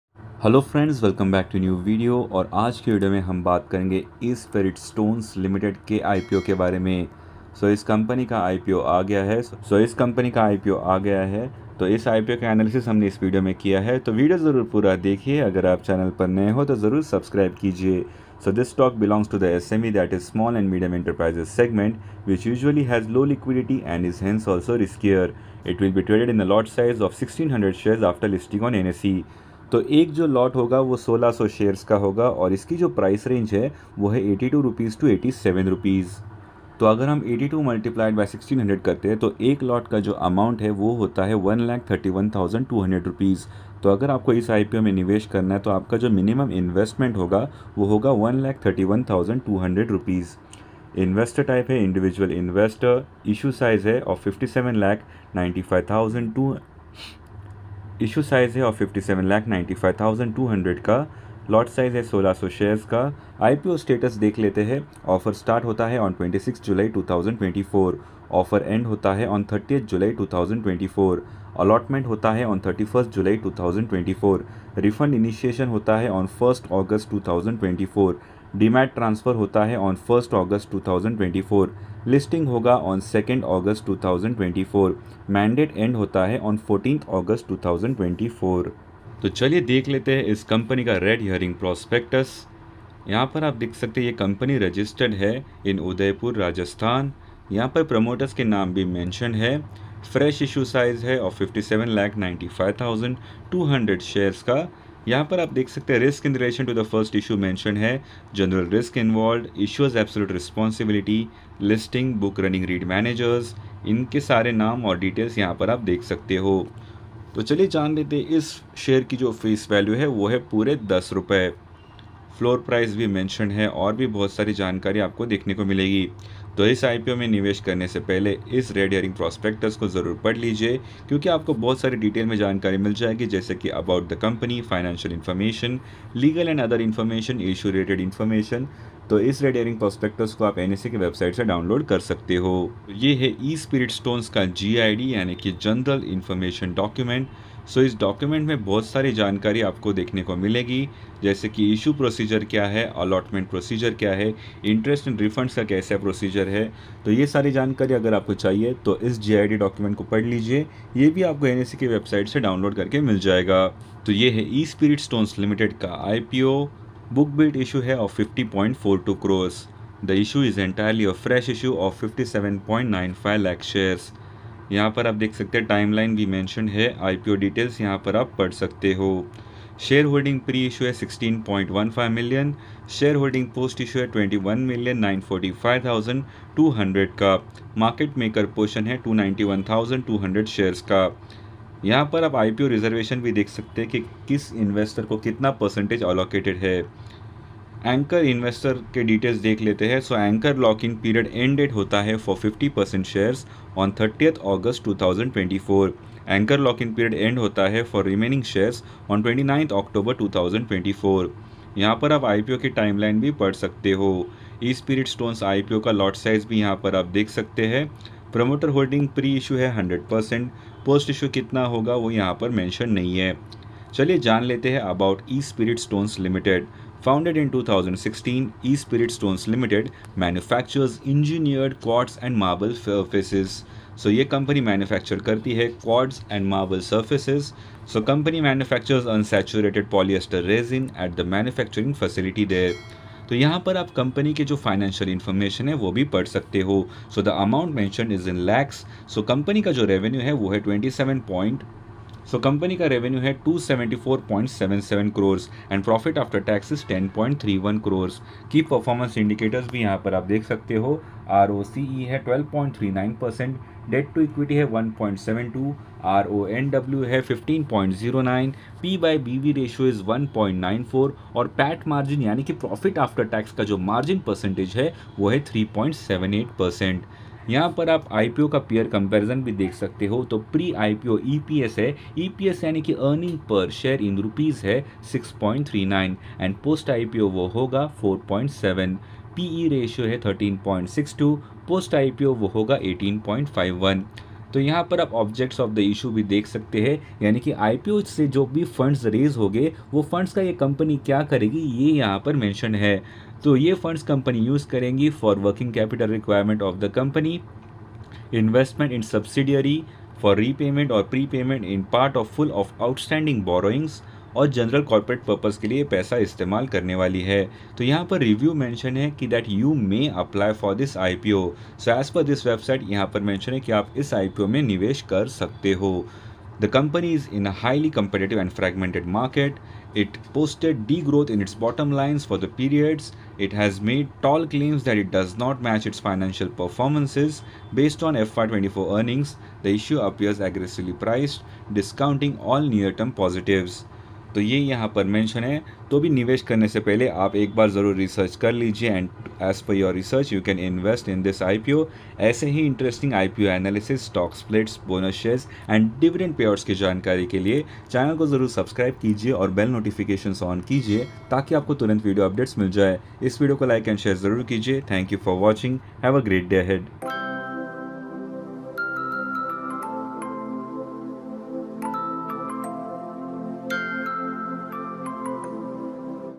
Espirit Stones IPO Analysis | Investor Goals
In this video you will get to know about Espirit Stones

This video only for education and knowledge purpose. Before taking any decision please do your own research. The channel would not be responsible for any financial gain as well as for losses. I am not SEBI registered.